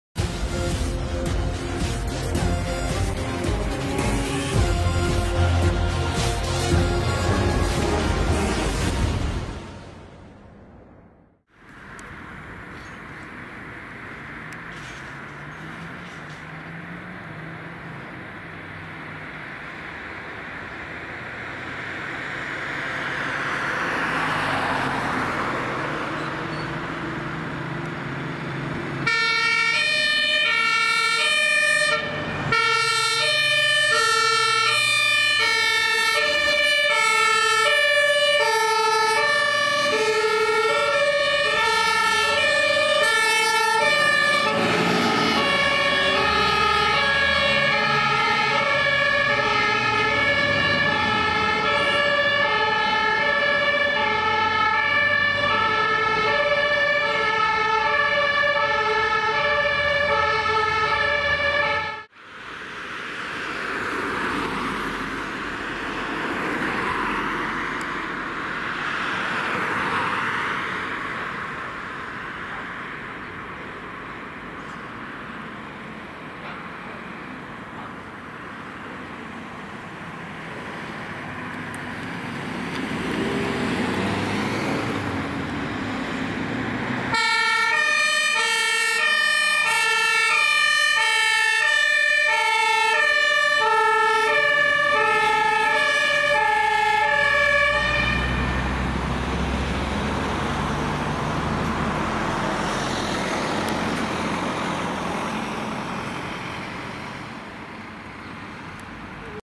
[Heimrauchmelder ausgelöst] Einsatzfahrt FFW Bad Doberan
In diesem Video zusehen, ist die Einsatzfahrt vom TLF4000, die DLAK23/12 und das LF20 von der Freiwilligen Feuerwehr Bad Doberan.

In meine Videos geht es um die Einsatzfahrzeuge auf Alarmfahrt. Personen sowie Gegenstände sind Beiwerke und dienen nicht im Vordergrund des Videos ⚠️.

Gefilmt mit: Panasonic HC-V 785

Datum:09/2023